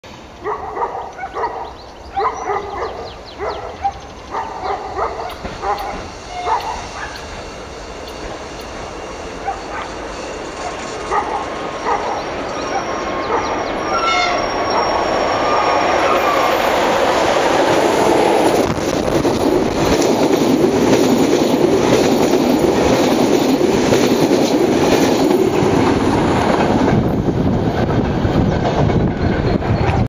OsR 30602 POHODA PÁRTY vlak do Trenčína
ZSSK dnes vypravila mimoriadny vlak z Košíc do Trenčína na festival Pohoda v čele s 362.008.
Ďakujem rušňovodičovi za pozdrav!
Stankovany 7.7.2016